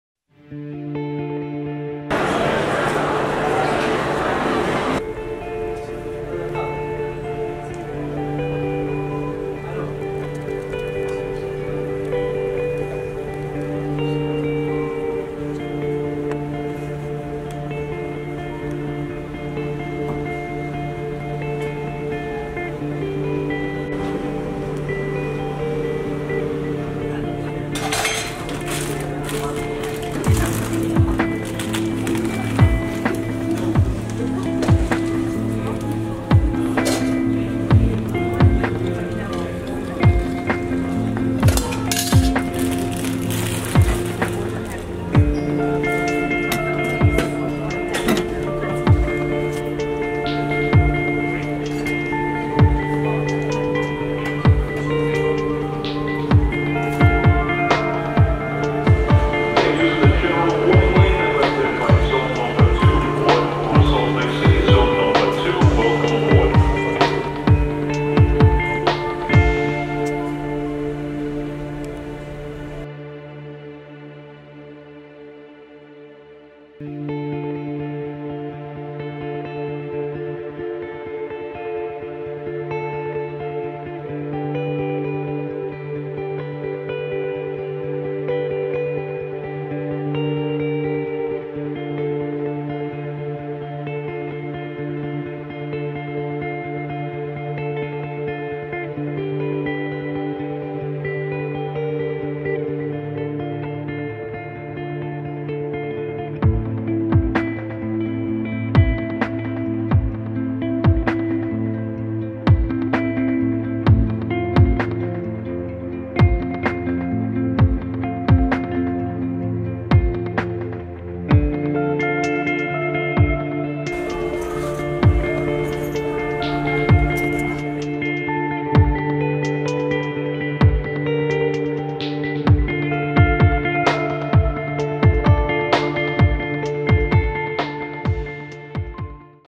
Delta Airlines Lounge @ New York JFK | Sky Club | Delta Business Class Lounge
Hi, here are some short impressions from the Delta Business Class Lounge at New York's JFK airport. It's not really a thorough review, but rather a compilation of footage that I took and decided to upload so that you can see how it looks like.

If you have time please check out my video about Delta's business class and my other videos on travel, business and technology.